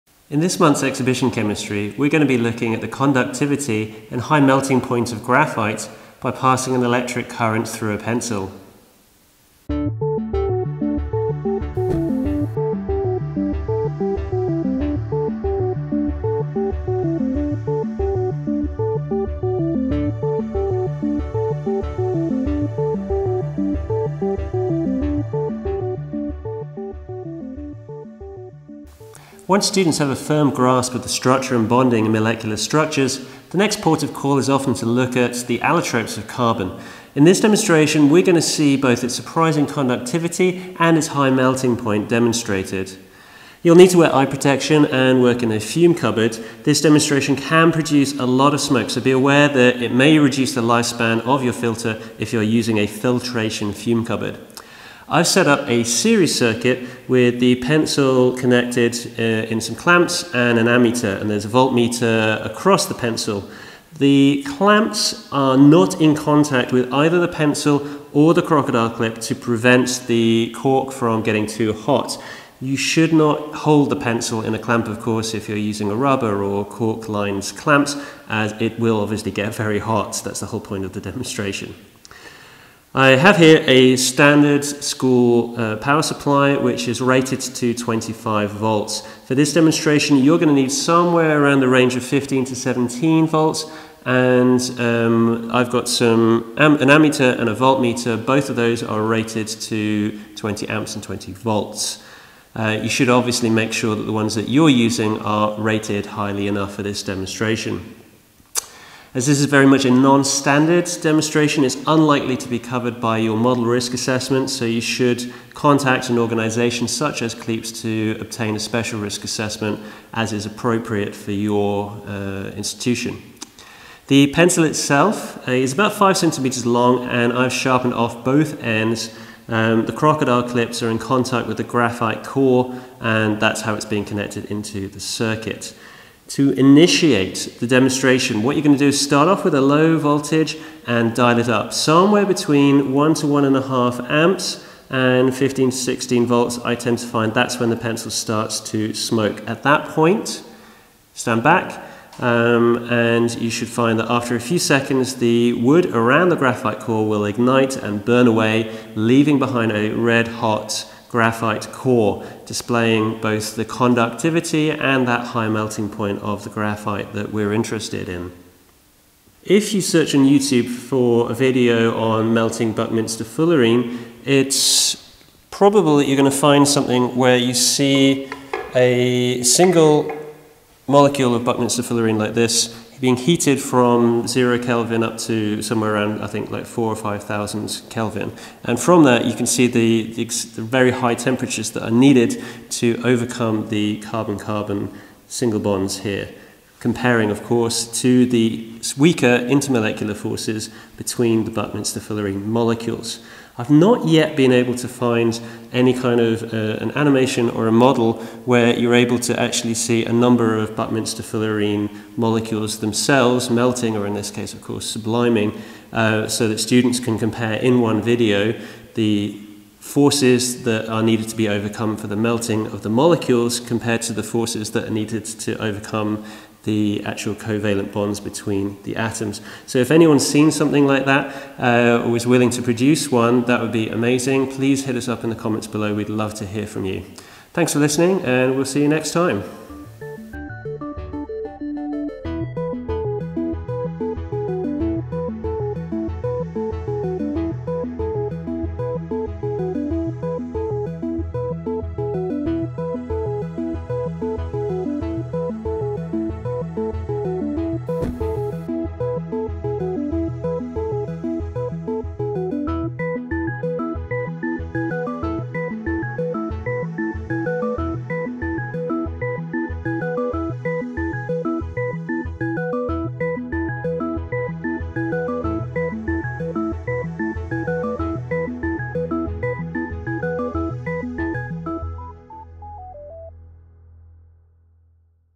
Red hot carbon – the structure and bonding of graphite
Teaching the structure and bonding of carbon allotropes can be a little dry; you can spice things up a little by showing some of the key properties of graphite, dramatically destroying a pencil in the process.

Read the article that accompanies this video in the May 2015 issue of Education in Chemistry

This is part of the Education in Chemistry Exhibition Chemistry series: chemistry demonstrations to capture your students' imaginations